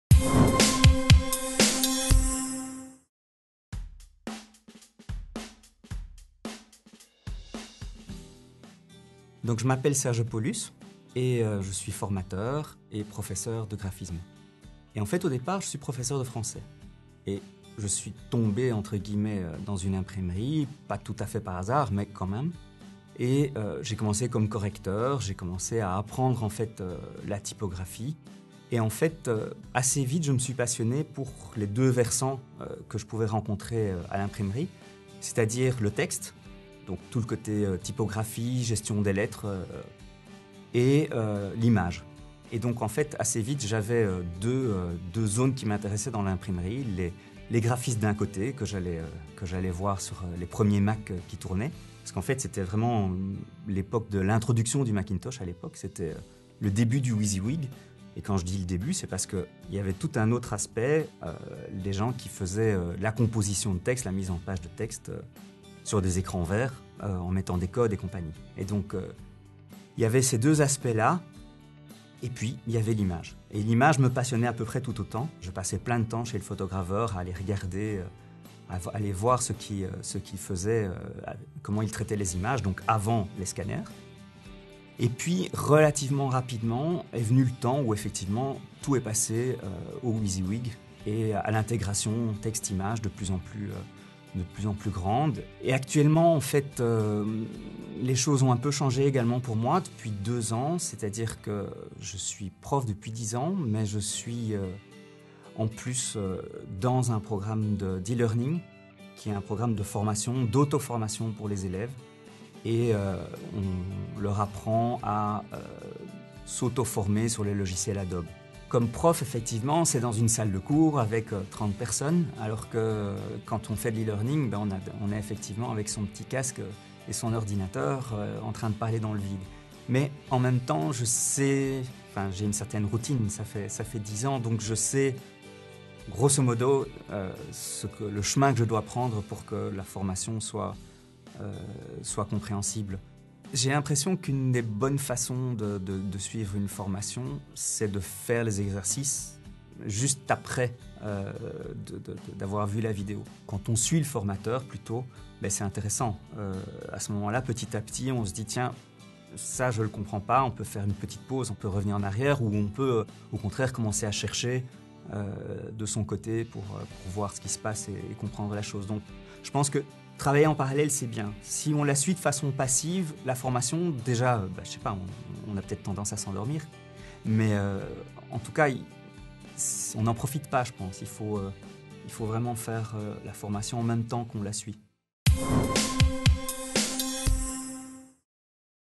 Interview de Serge Paulus | video2brain.com
Serge Paulus a été professeur de français et a ensuite travaillé 15 ans dans l'industrie de l'imprimerie et du graphisme à l’époque du passage au Macintosh, au numérique et au WYSIWYG. Il a décidé de partager son expertise en technique de design graphique en classe et est retourné dans l'enseignement en école supérieure et artistique.

Il est depuis 2013 formateur certifié Adobe en Photoshop. Actuellement, il crée des didacticiels d'apprentissage en ligne sur les logiciels de graphisme d’Adobe, pour l'école de communication IHECS à Bruxelles. Il est à cette occasion devenu Adobe Education Leader.

Il enseigne également l'utilisation des logiciels de graphisme et de conception de polices de caractères dans le département Bande dessinée et édition à l'ESA Saint-Luc à Bruxelles. Profondément investi dans la typographie, il conçoit des polices, et aide ses élèves à créer la leur.

Passionné par la photographie, il a donné de nombreux cours à des photographes désirant passer au numérique ou maîtriser Lightroom et l’ensemble de la chaîne graphique, via des ateliers pratiques.